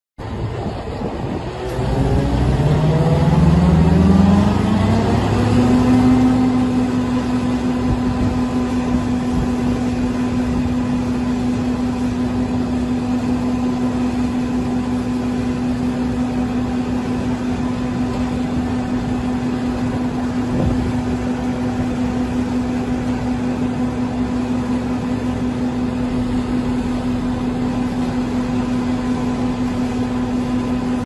1983 GREAT DANE VAN TRAILER For Sale
1983 Great Dane Van Trailer, Converted to silage trailer, Belt system pushes the silage out the back, Tires 11R22.5, 5thWheel Plate was replaced.